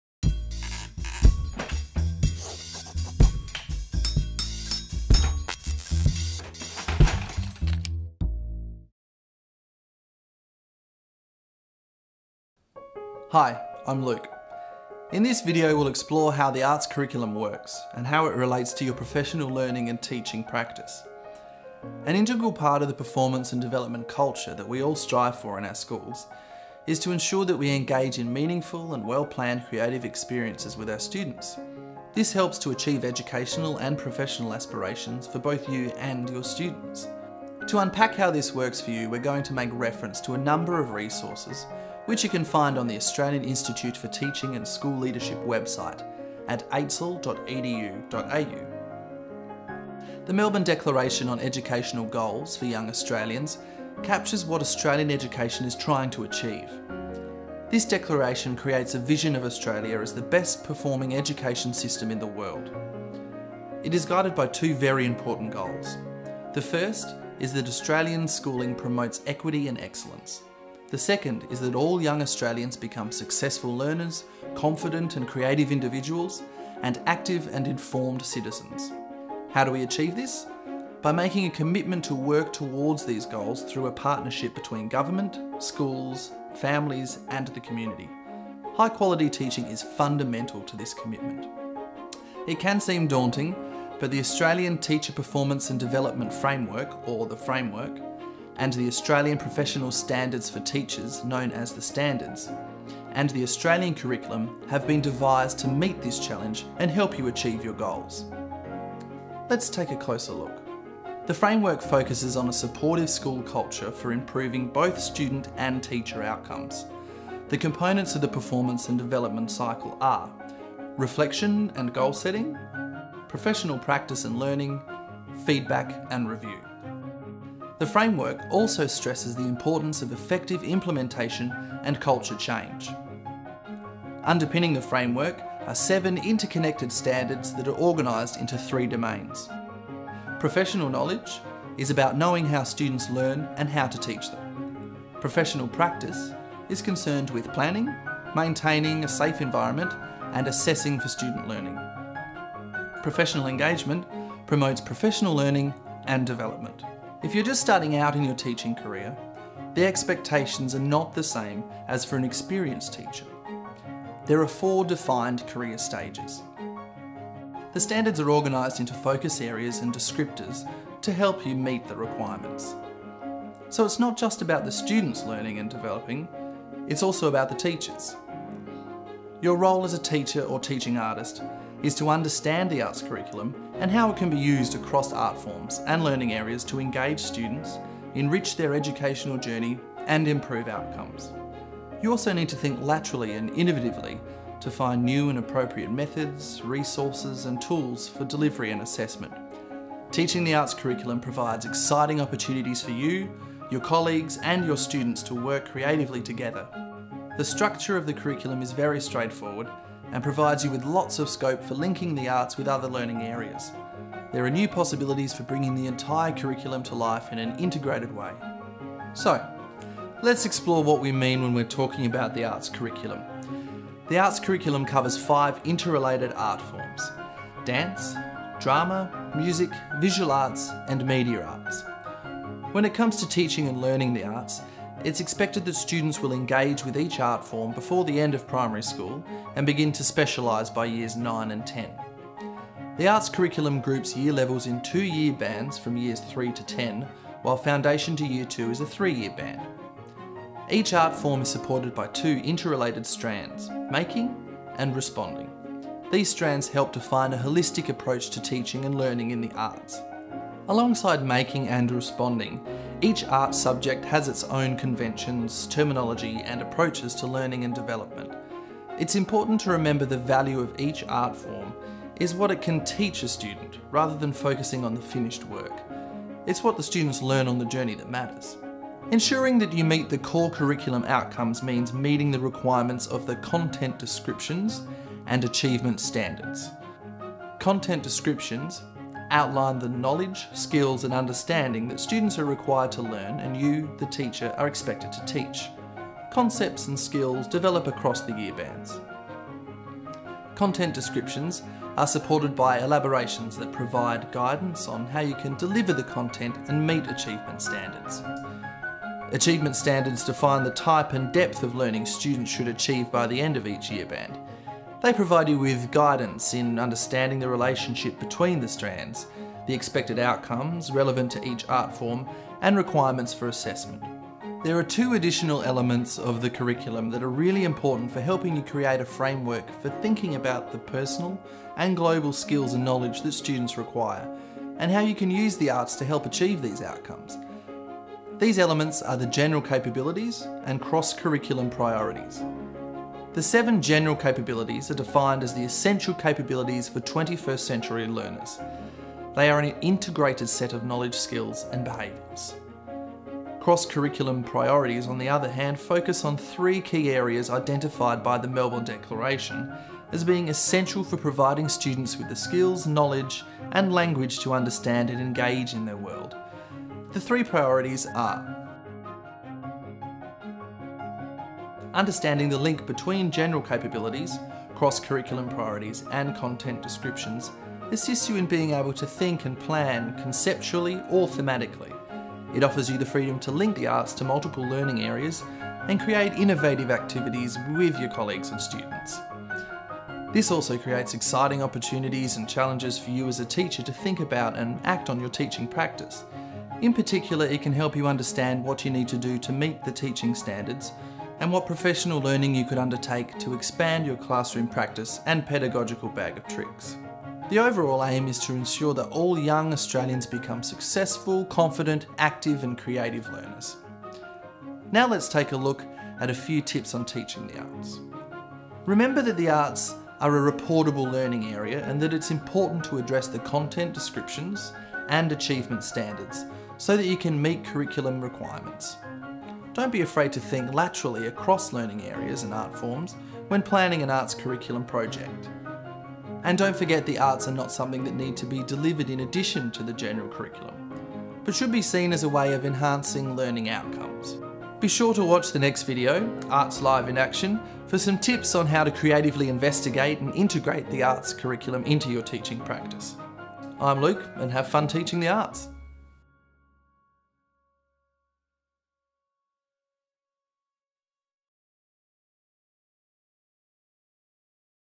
The Arts Curriculum and Teaching Practice
This video, created for the Song Room's website ARTS:LIVE, gives an in depth overview of the Australian Curriculum for The Arts.

Or if you are interested in having a video like this one created,